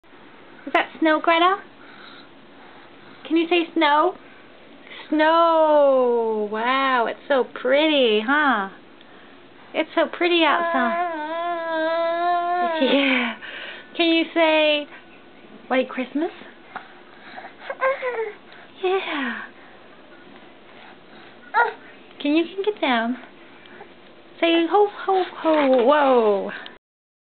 First snow of 2009 - Saturday, December 5th!
Greta at 14 months enjoying the winter wonderland (inside).